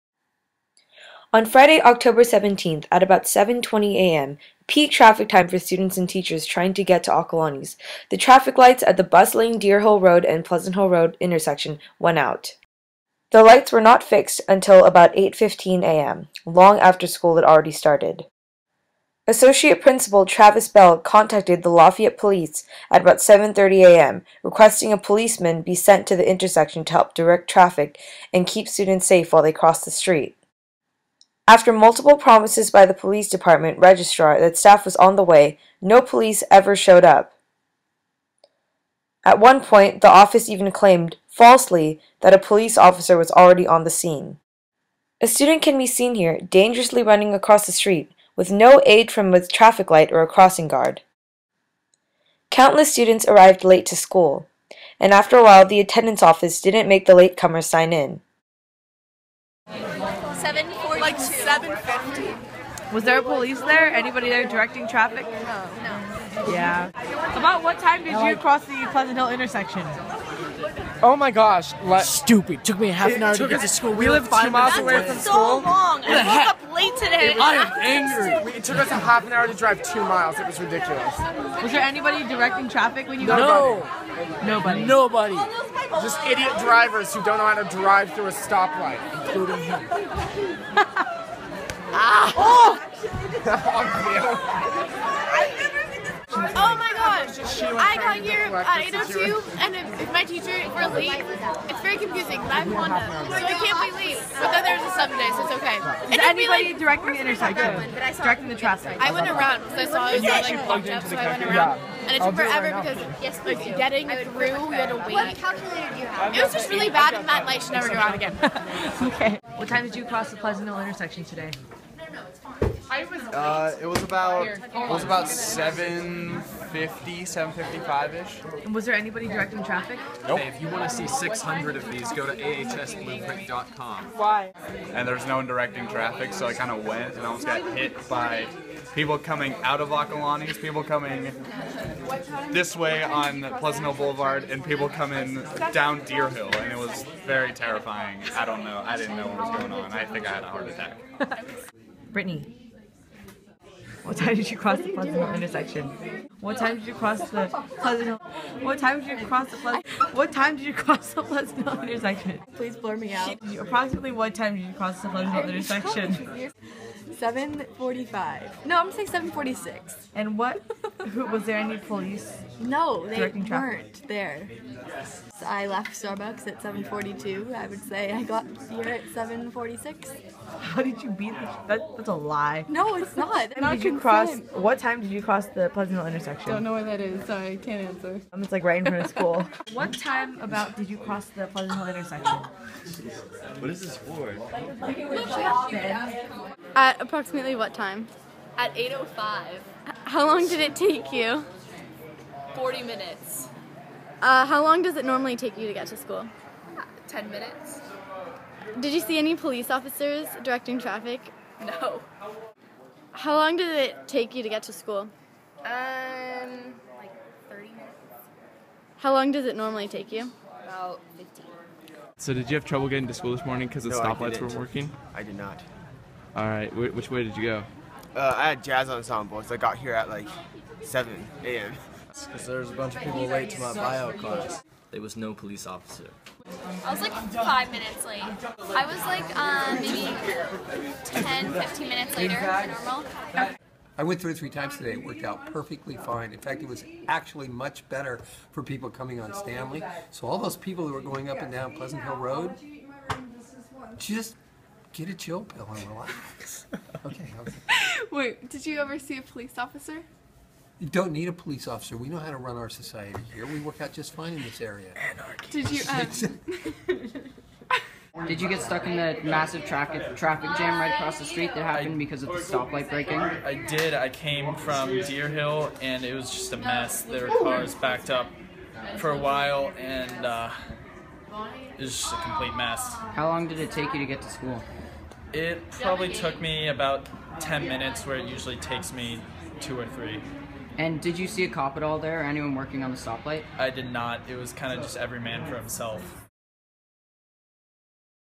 Lafayette Police Fail to Respond to Traffic Light Malfunction
Traffic sucks.